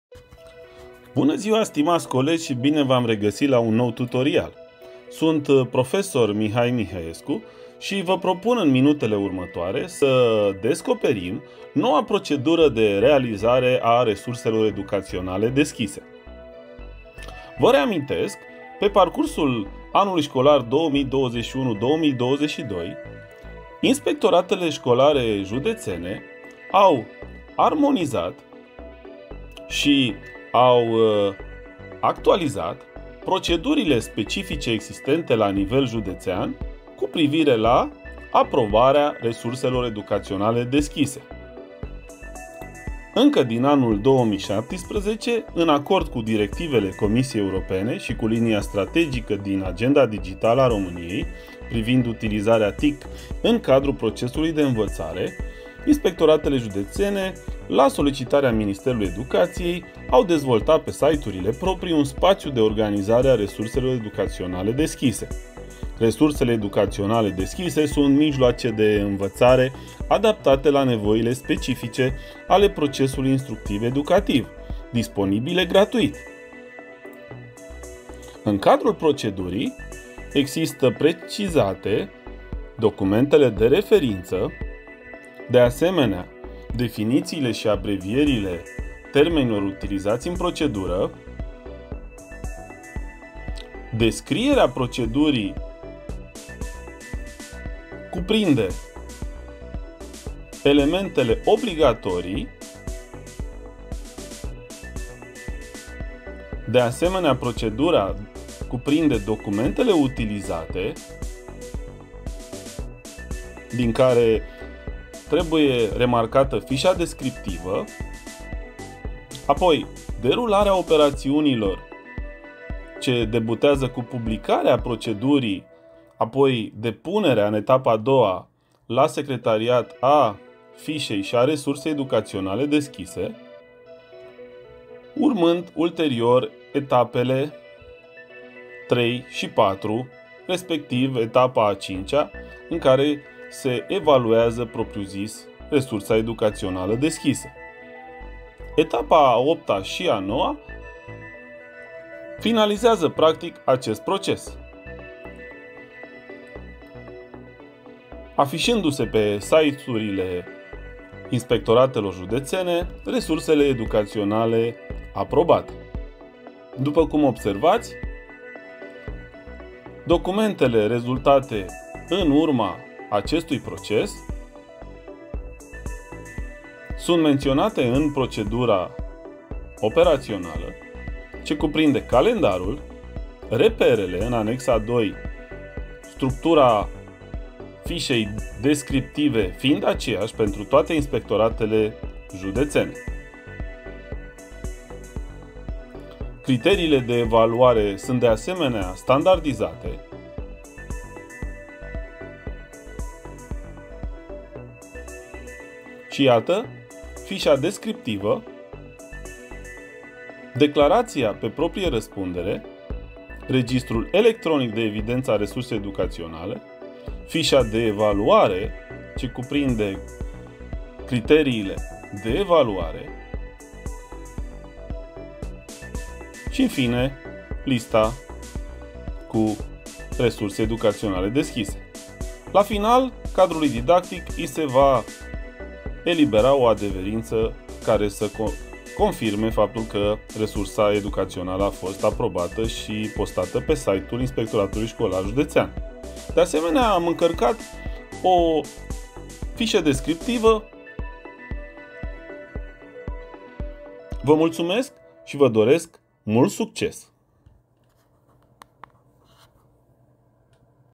Procedura RED
Scopul resursei este de a susține activitatea didactică, mai precis o colectare a resurselor deschise și o prezentare a acestora ce poate fi inclusă pe site-uri ale unităților de învățământ sau pe platformele acestora.
Cuvinte cheie: resurse educaționale deschise, site,elearning etc.